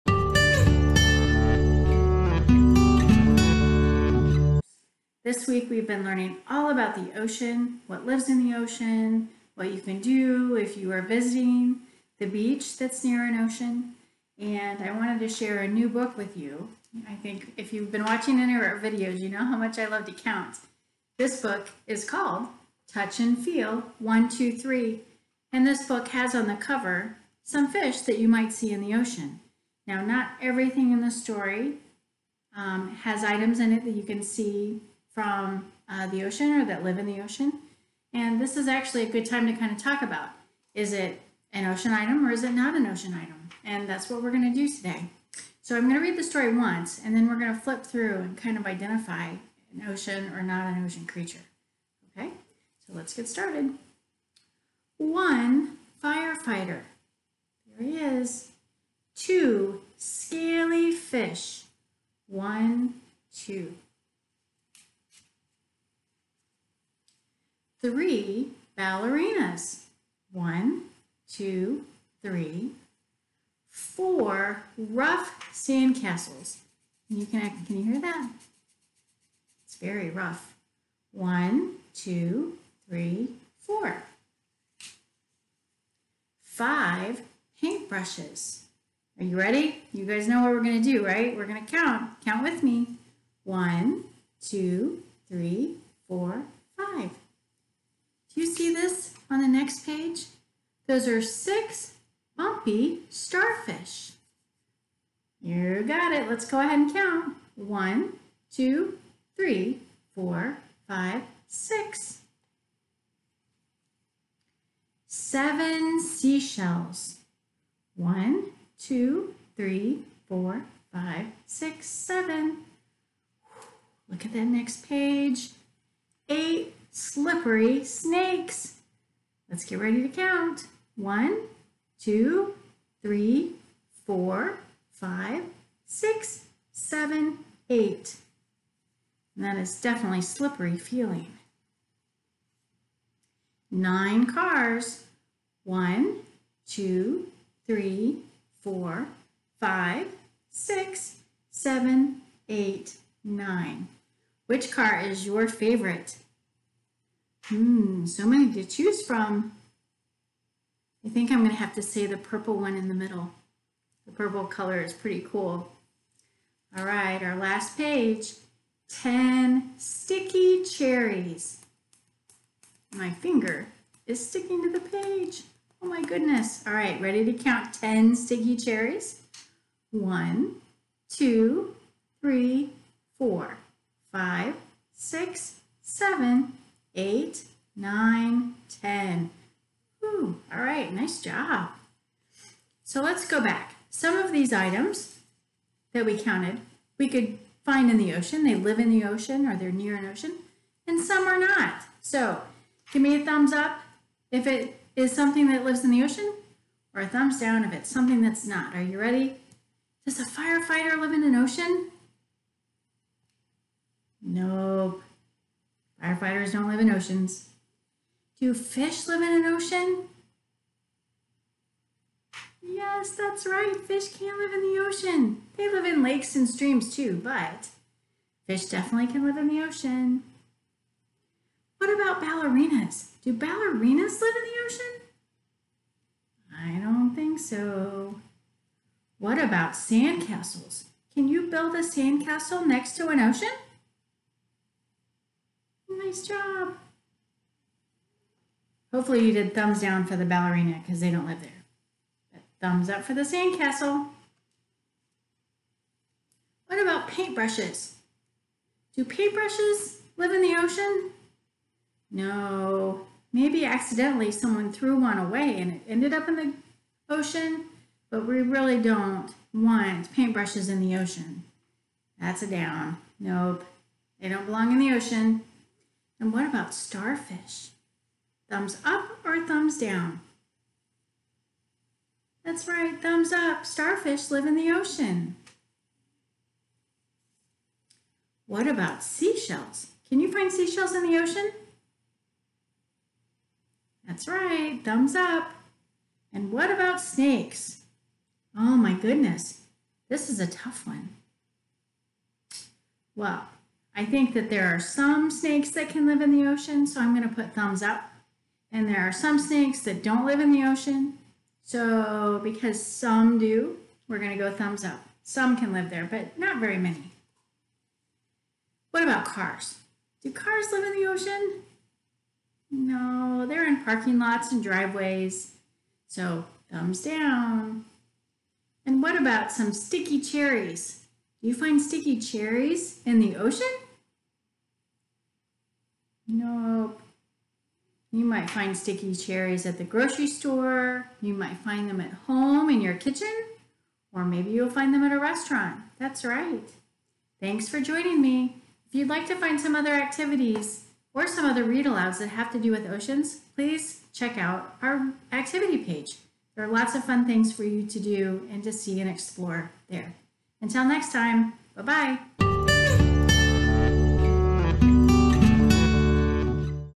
Reading Touch and Feel 123 with Ms. Michelle
Ms. Michelle reads Touch and Feel 123. for more information!